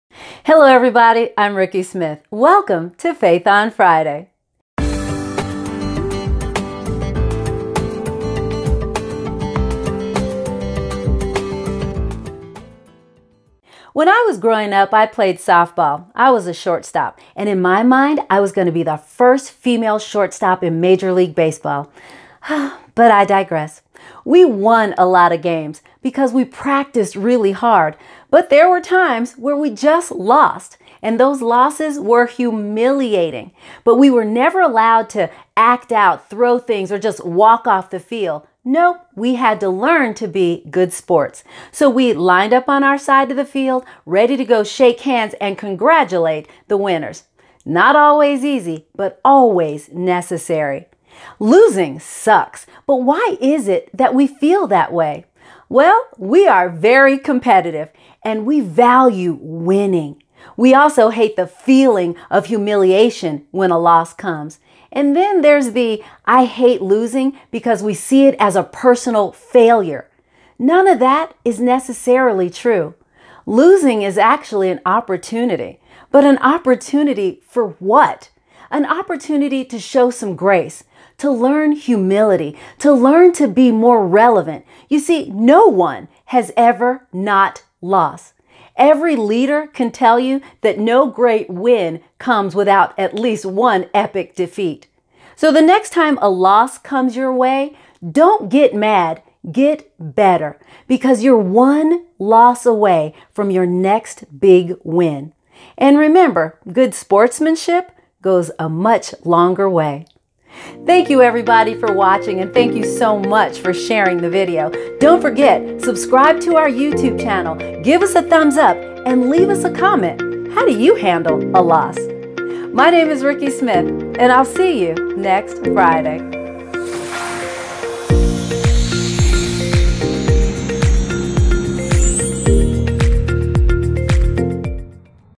Faith on Friday - Losing Sucks
It's never fun to lose. When you put your heart, soul, sweat, and tears into something and then not be the winner...UGH!!! Losing doesn't have to be the end, it can actually be a beginning. when you fall short of the goal, reevaluate the process, ask questions, and start again. Yeah, losing sucks, but it can also make you better.